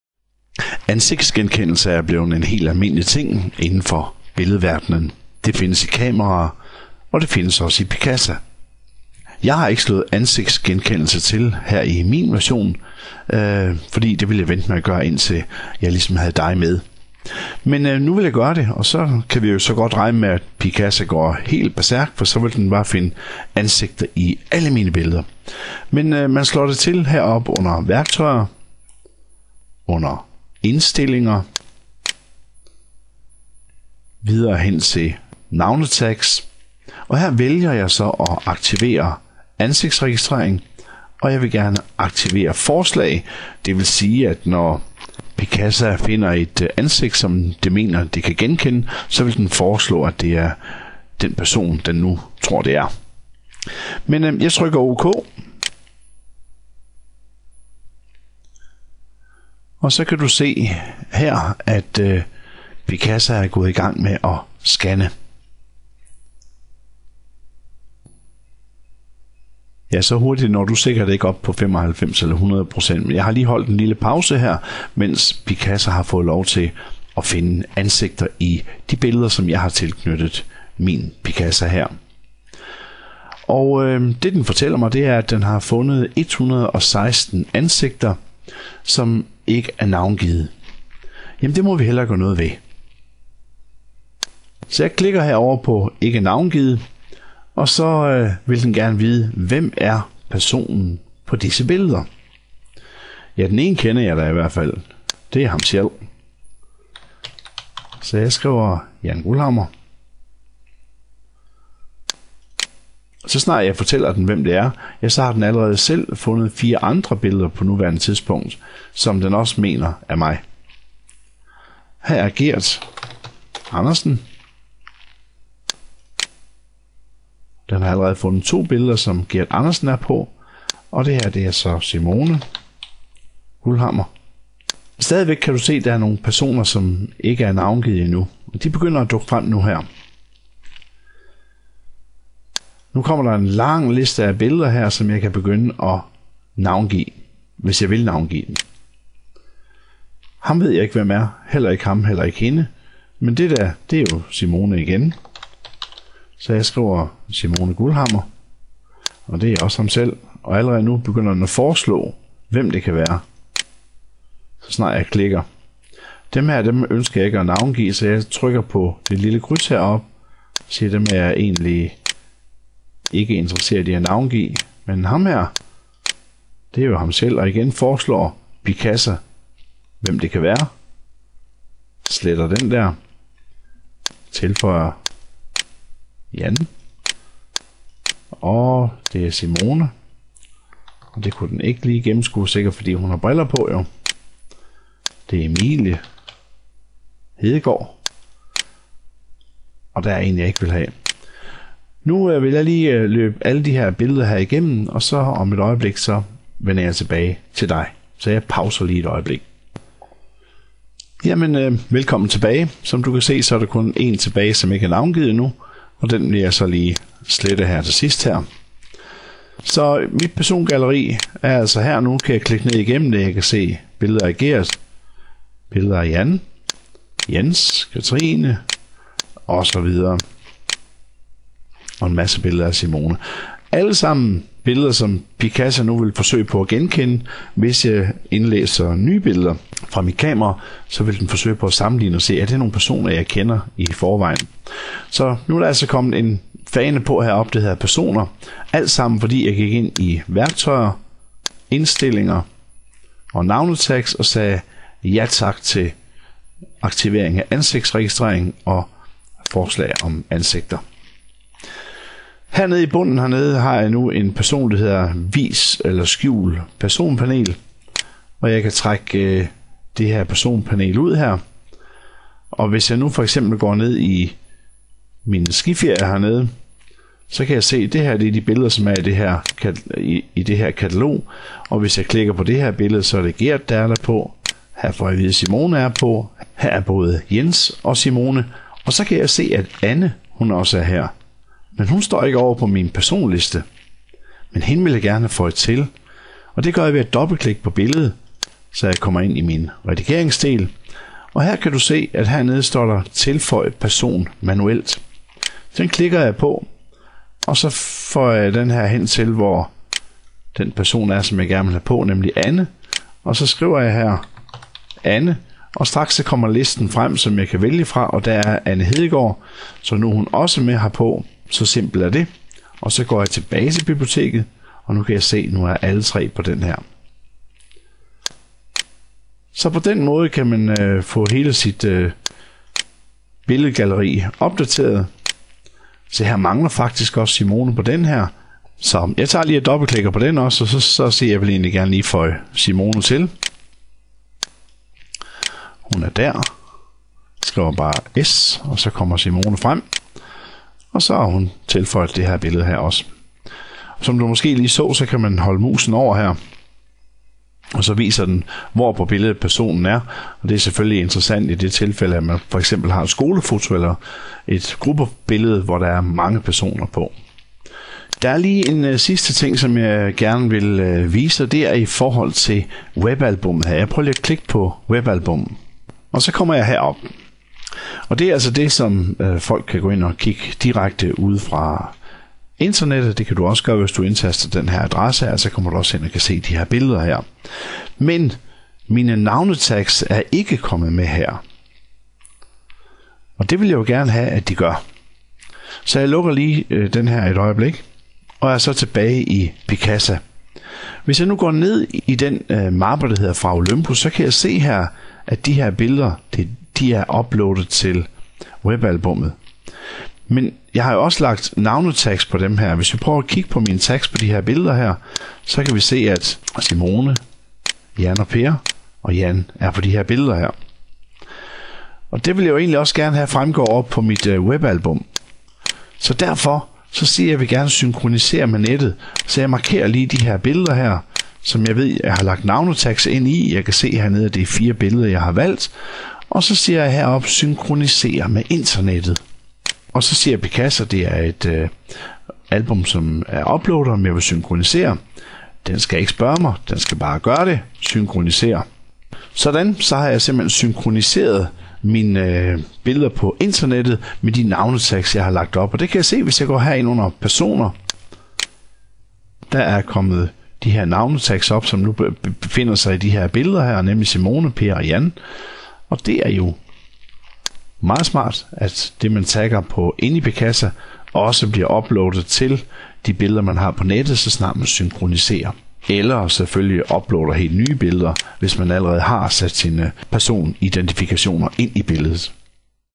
Picasa ansigtsgenkendelse
Ansigtsgenkendelse i billeder er en smart funktion der gør det let at finde bestemte personer i sit billedarkiv. Også i Picasa er denne smarte funktion implementeret.